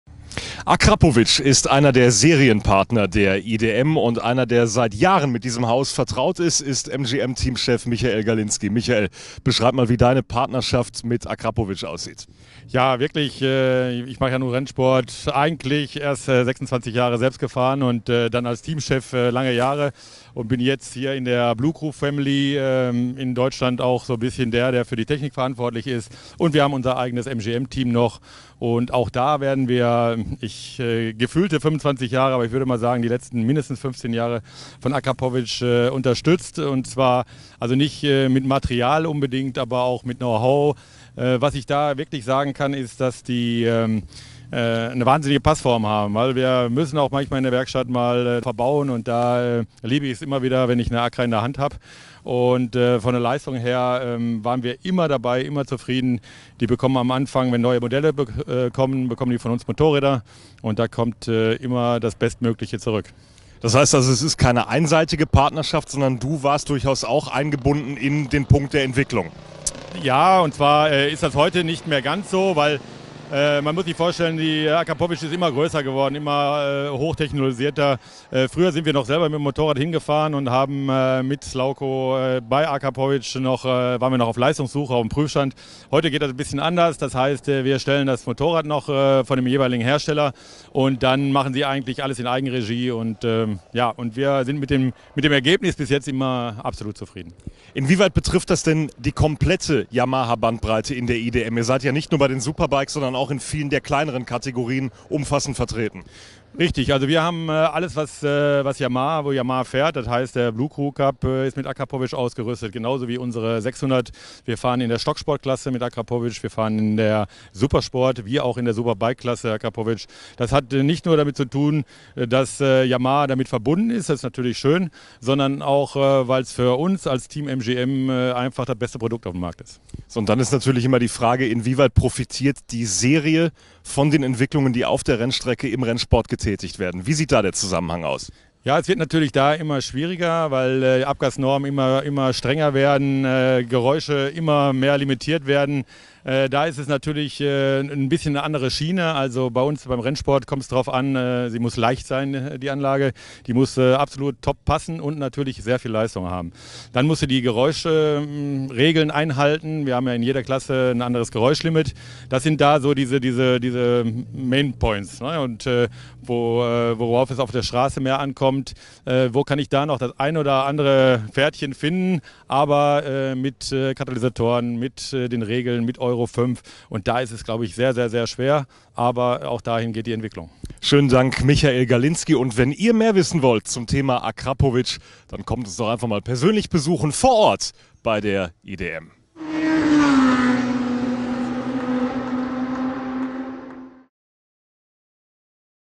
IDM 2019 - Akrapovic @ IDM
Michael Galinski, Teamchef von MGM Racing Performance, im Interview mit Lukas Gajewski über die Auspuffanlagen von Akrapovic.